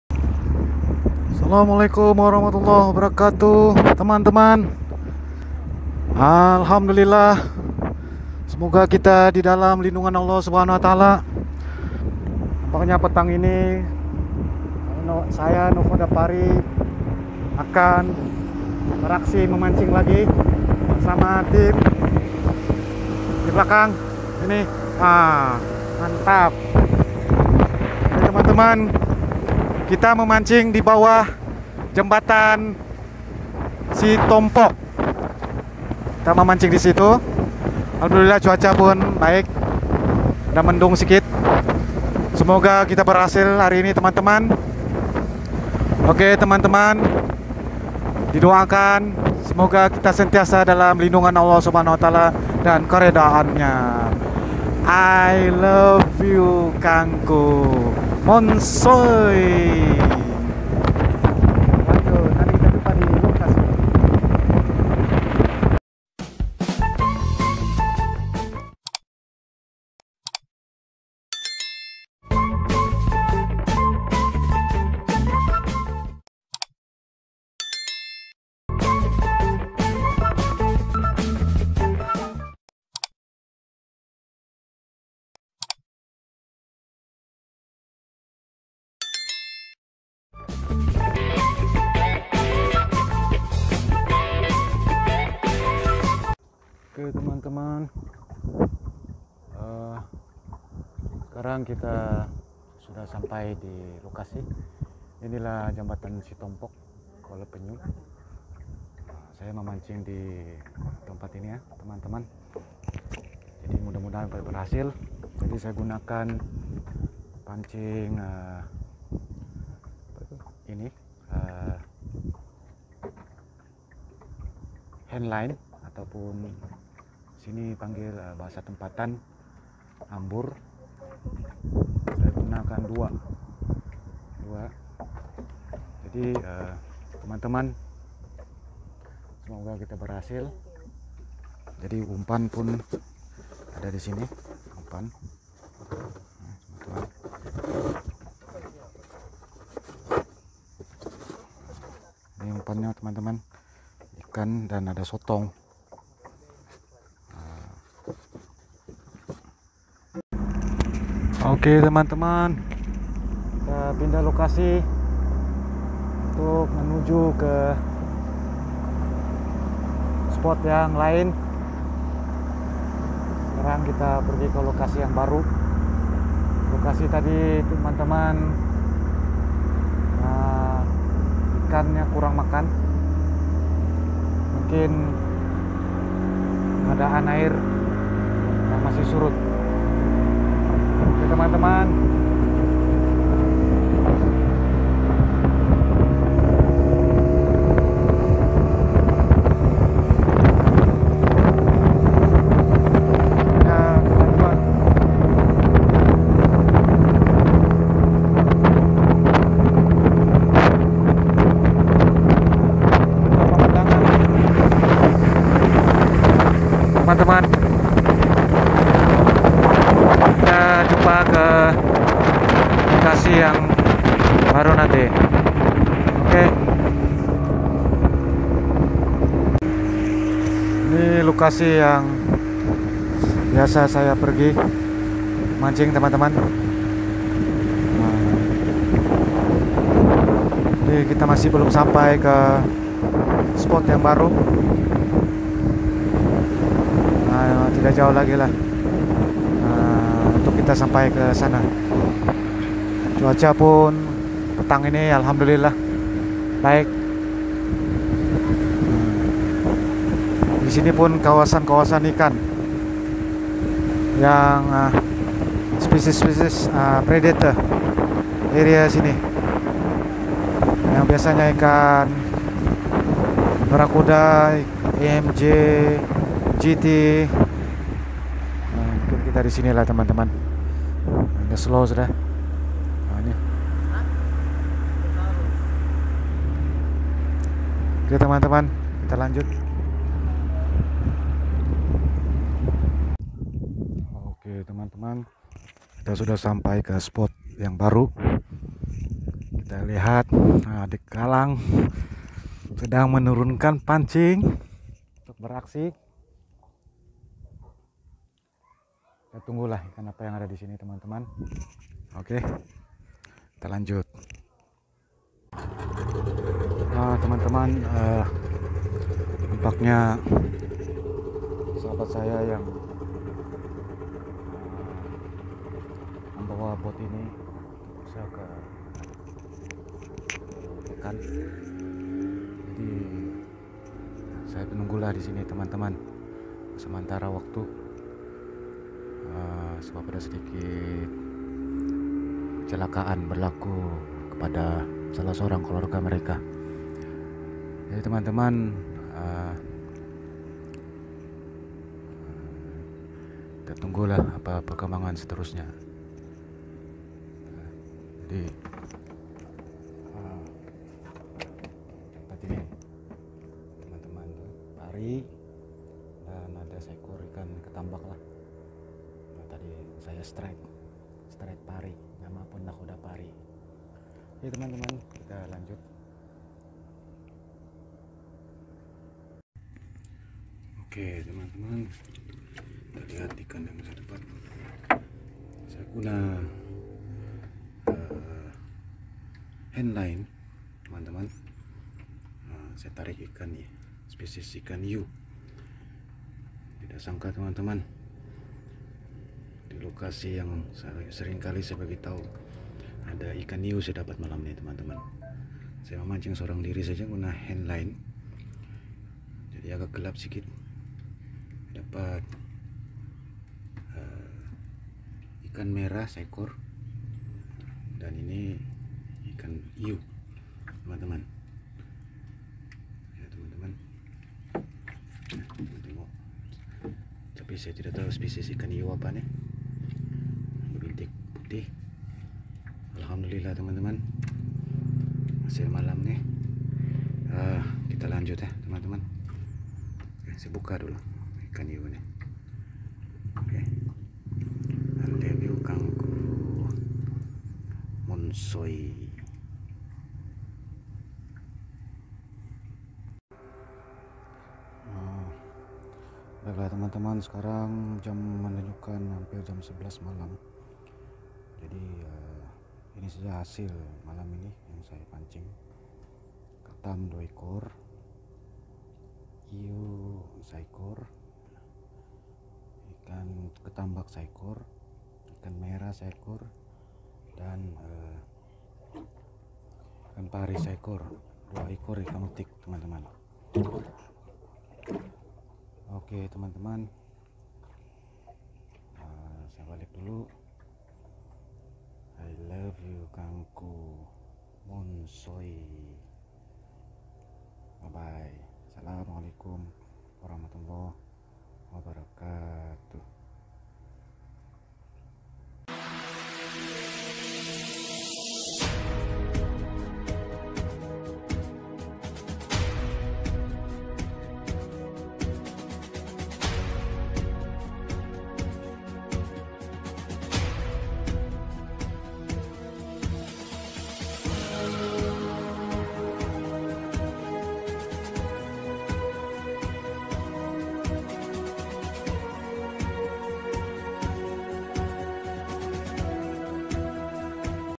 Ep14// Memancing Ikan Di Pangkalan Kapal Strike Ikan Yu
Sinopsis Tempat Saya Spot Memancing.

Sebelum ini saya ada memberikan info mengenai lokasi spot memancing bilamana kawasannya adalah secara umum didiami oleh ikan2 pemangsa (predator). Seperti yang teman2 lihat divideo yang saya upload itu ikan yang berjaya saya pancing dgn menggunakan handline (ambur/jaul). Spesis ikan yu yg berjaya saya pancing adalah salah satu penghuni di teluk sitompok selain daripada ikan pemangsa sprti MJ/GT/Baracuda/Grouper Dan Tenggiri.
Semoga teman2 terhibur menonton video aksi2 saya memancing. Aksi2 memancing video2 akan dtg menyusul sebagai santapan santai menonton Youtube Channel NAKHODA PARI.

Terima Kasih Sokongan Dan Dokongan Teman-Teman Semua

Wassalam😎